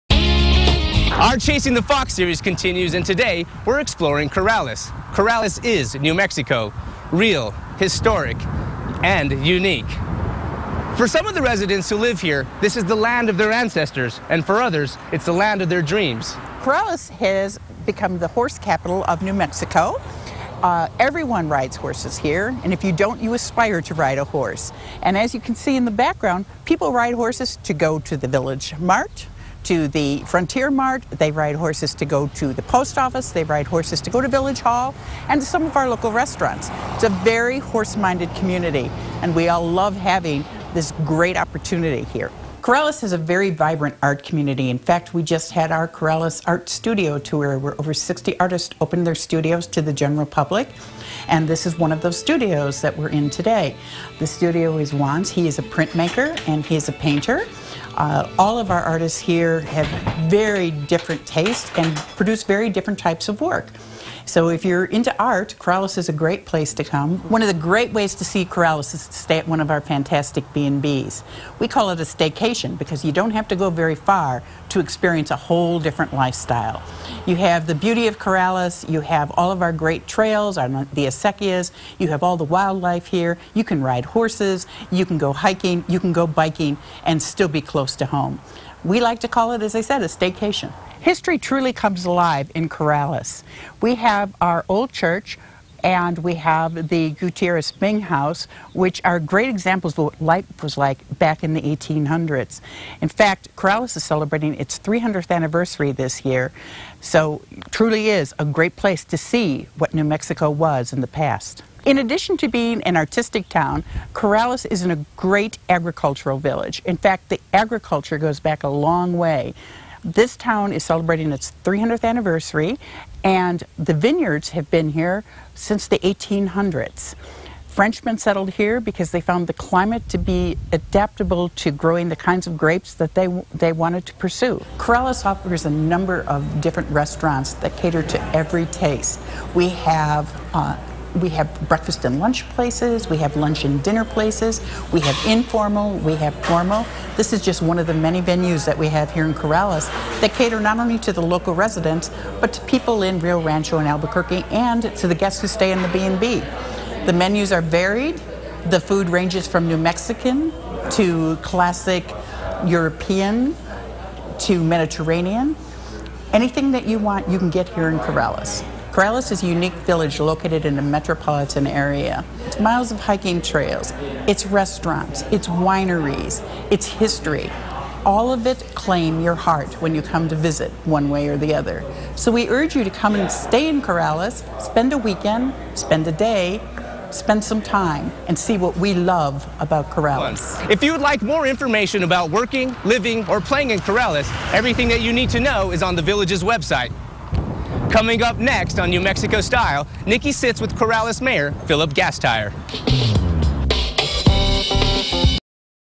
Chasing the Fox: Corrales
The "Chasing the Fox" series continues in Corrales, and Face of Fox, Elias Gallegos, shares ways to enjoy the "Horse Capital of New Mexico." Judith Newby, of the Sandhill Crane Bed and Breakfast, shows off how the community exhibits their pride in Art, their agricultural village, and delicious and diverse restaurants.